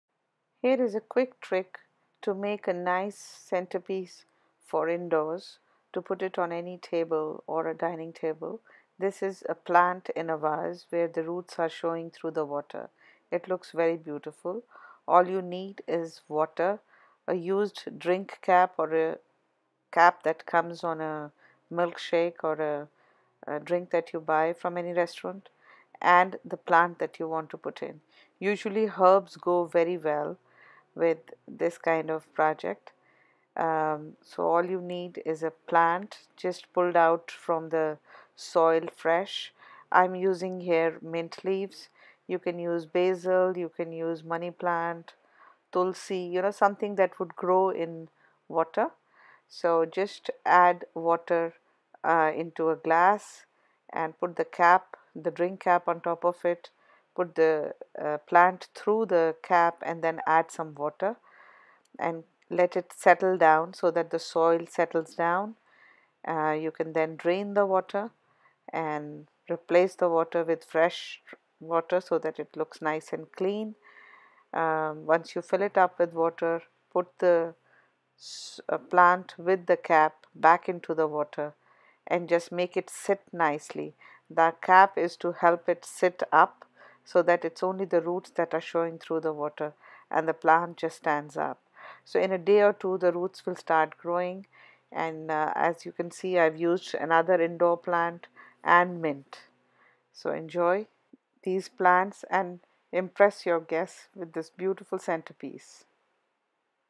Plant in water
Green looks beautiful indoors, more so if it is in water. Here is a nifty trick to being some greenery inside your home on your kitchen counter or into your family room. A green herb or indoor plant with its roots showing in clear water not only adds vibrance to a room but also has other benefits. Like a mint twig in a room keeps bugs and mosquitoes away. Easily available indoor plants like a creeper, English Ivy, bamboo plant or money plant in water adds beauty and freshness to any room decor.

You need minimal materials for this project. A glass vase, water, a plastic drink cap and a plant. Check out the video above to see how to get through with this quick project.

Handy Tips:

1. Make sure you do not leave any detergent residue in the glass vase where you want to put this plant as it may result in developing bacteria that may kill the plant.
2. Do not change water often as all algae that has developed will be lost. Algae helps keep plant healthy.